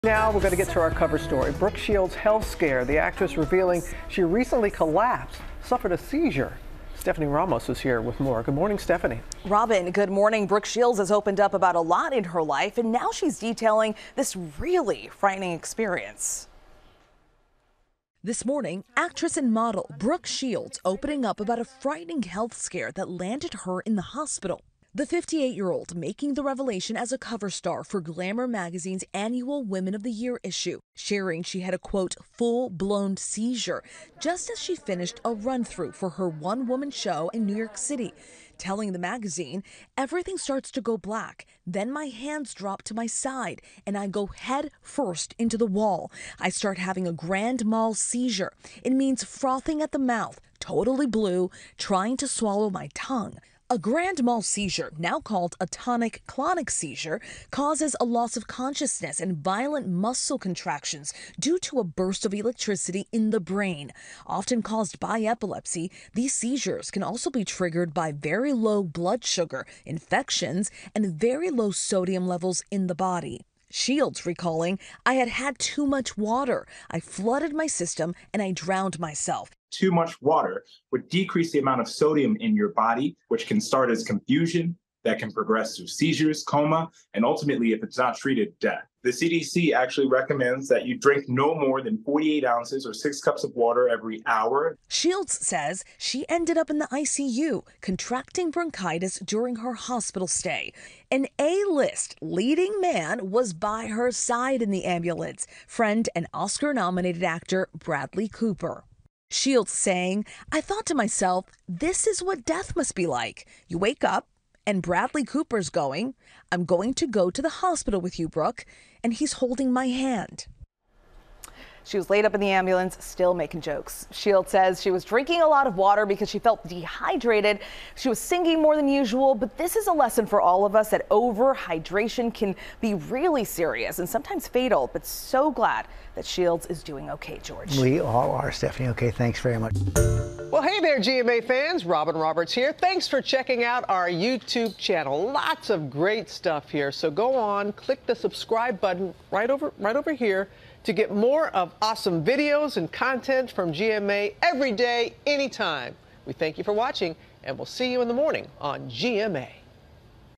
Brooke Shields reveals she had a grand mal seizure
The actress, model and author revealed she recently experienced the medical event, which sent her to the hospital.